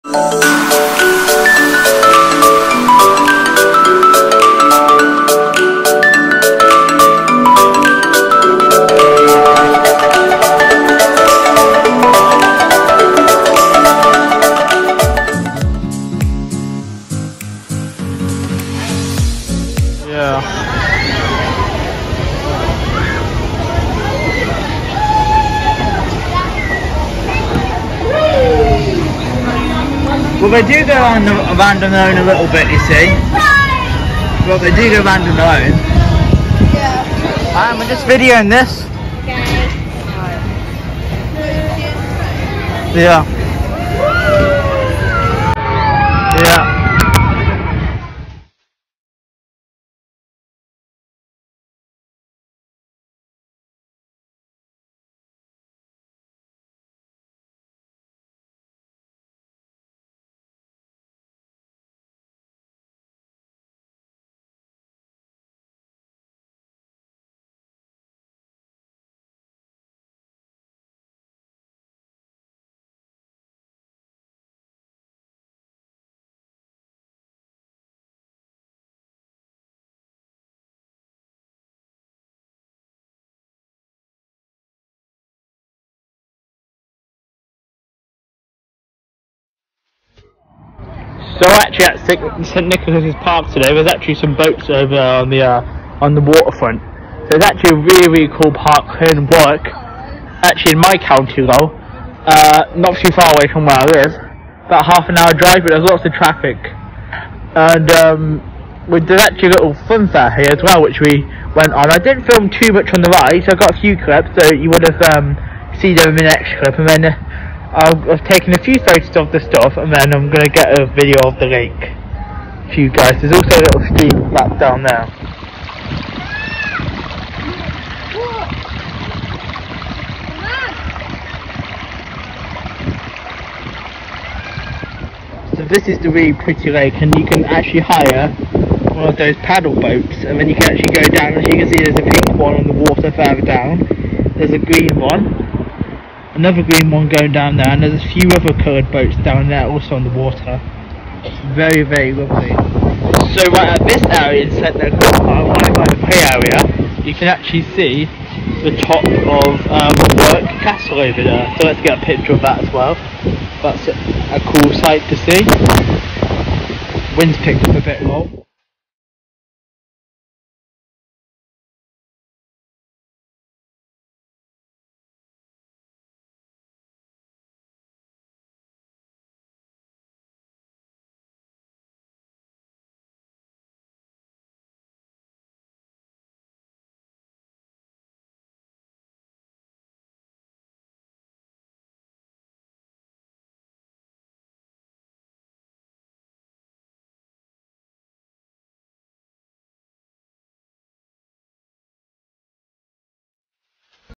St. Nicholas Park Warwick Vlog | August 2021
In Today’s video I film vlog of all the features of St. Nicholas Park which is a park located in Warwick, England this park includes The funfair,  the views nature + more and I show some photos too Troughout this video.
Date filmed: Friday 13th August 2021
Location filmed: As Listed Above!
Date Published: Friday 27th August 2021
Notice:
This is 2021 copyright of Sam's Vlogs trains & more please do not redistribute without consent, many thanks.
Chapters:
Cheers!